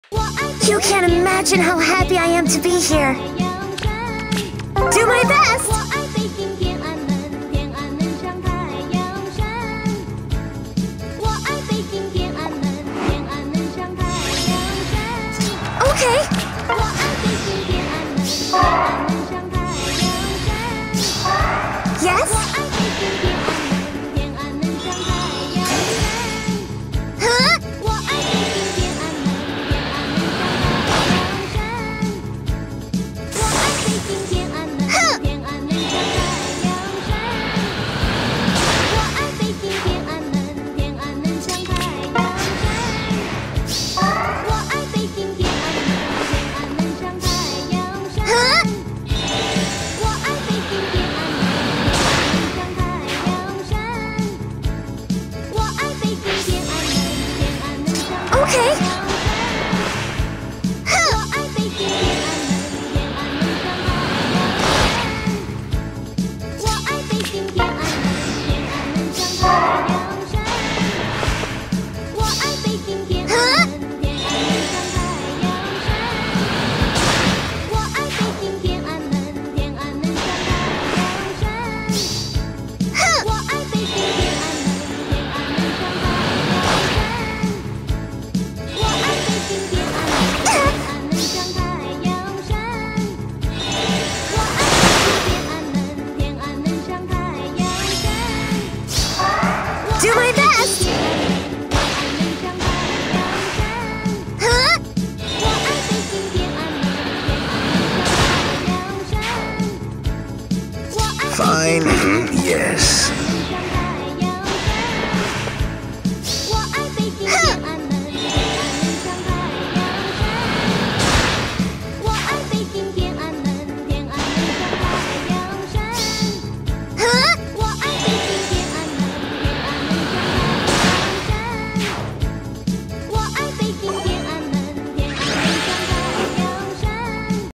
Infernal Oliver: Nino True Solo
Pretty much only bothered to record this because it's Oliver, and he deserves the attention. Not a good map.